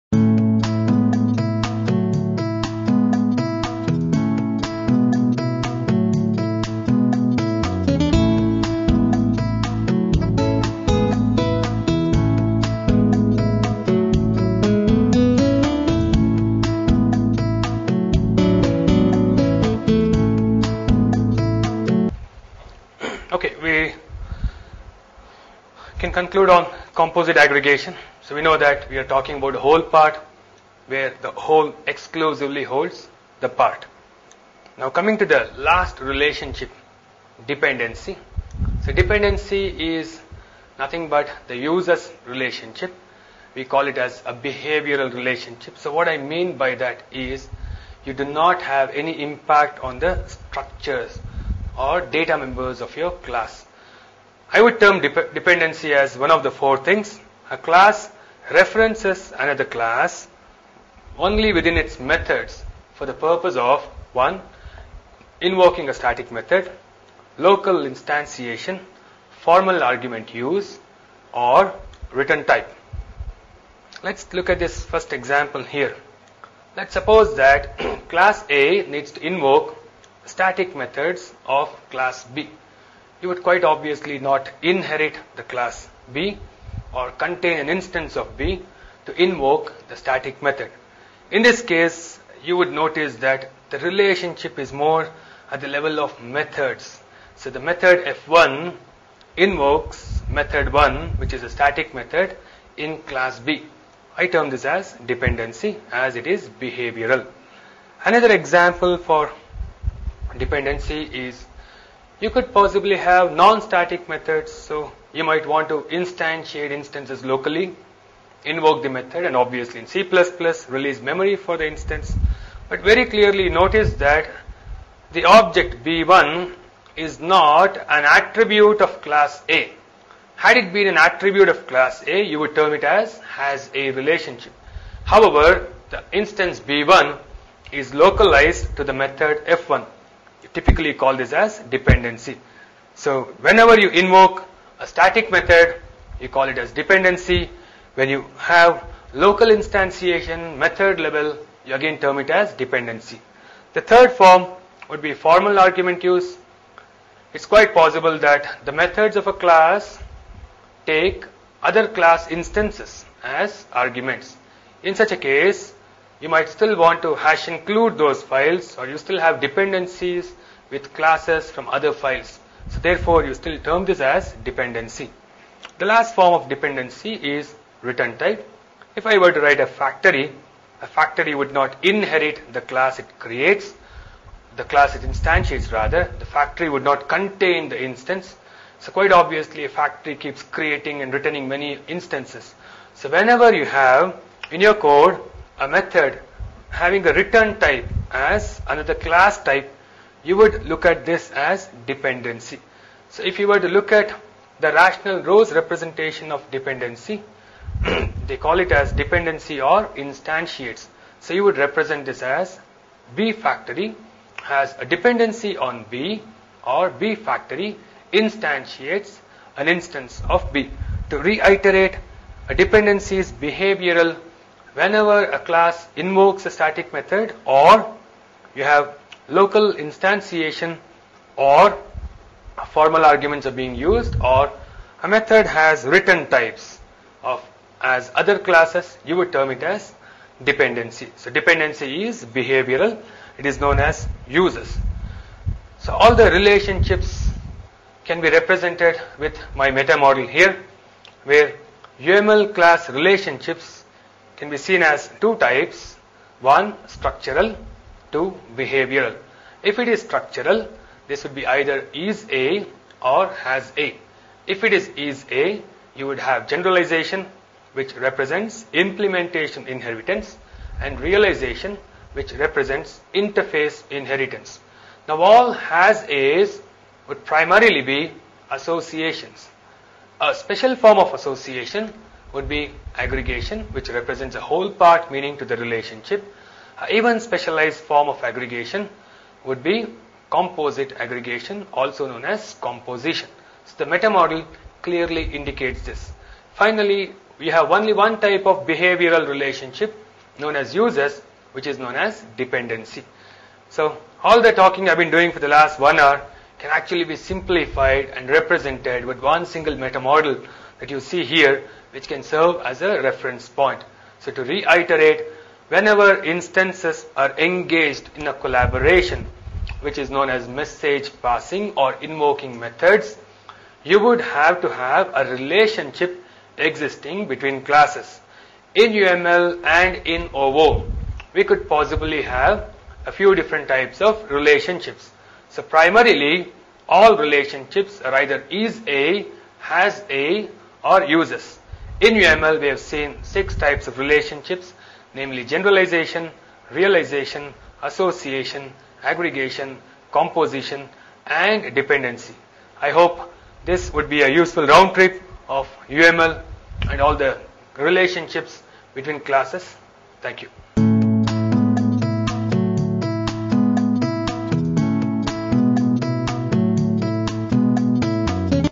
UML
This video course addresses the finer aspects of Object Oriented Design, using Java/C++/UML as the medium to reveal the power of Design Principles & Patterns. Each pattern will be discussed in detail, followed by an illustration using sample code and an analysis of implementation issues. The training will drive good OO principles and design techniques before patterns are discussed. A sound understanding of these principles and the exercises provided would be instrumental in ensuring a comprehensive understanding of Design Patterns. The program will be made as hands on as possible. Each pattern will also be modelled in UML with a Case Study that would be provided to the participants.